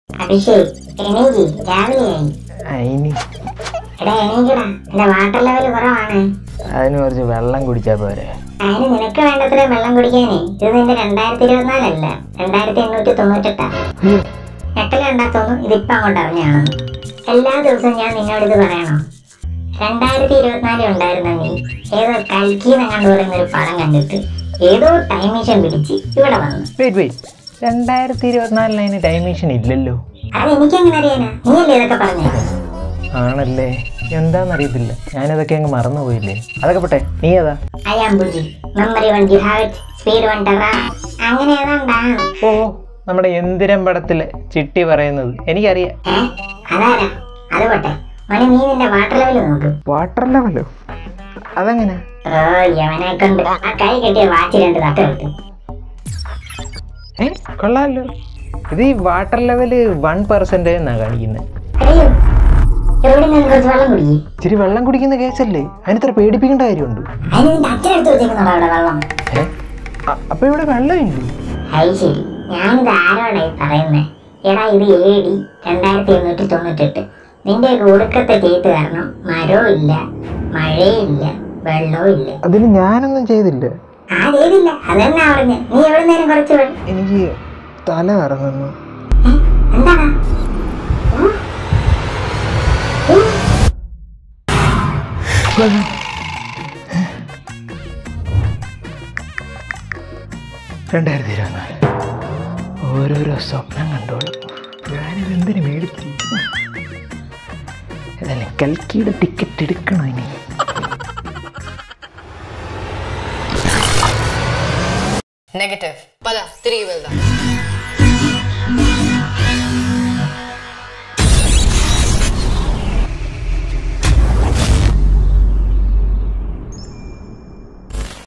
Kalki 2898 AD Low Budget | No Prabhas | No Kamal | No Deepika | No Amitabh | Abishek's Imaginations
Recreating Bujji from Kalki Cinematic Universe

Written & Directed by: Devanarayanan K.S & Abishek P Das